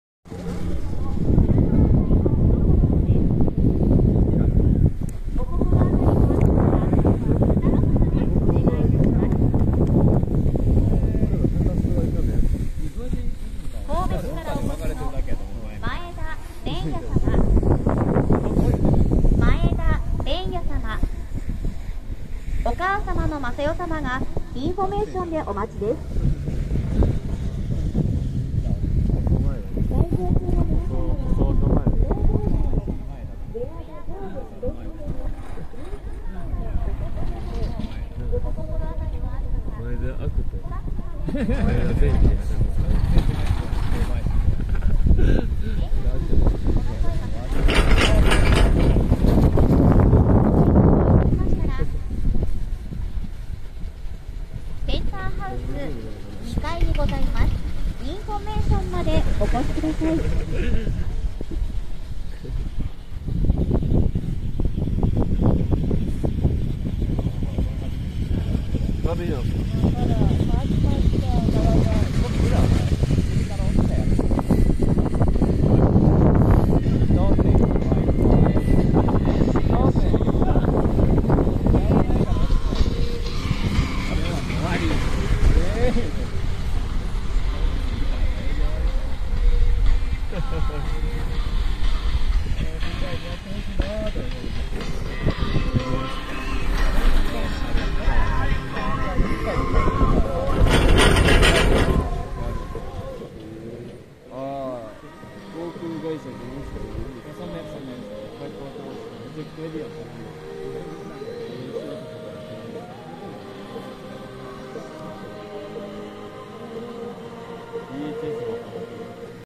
スキーバッジテストをダイナランドで受けたら神中Bが不整地小回りかなあ。3月11日12時23分気温10度
このチャンネルは高鷲スノーパーク&ダイナランド&スキー&スノーボードの専門チャンネルす！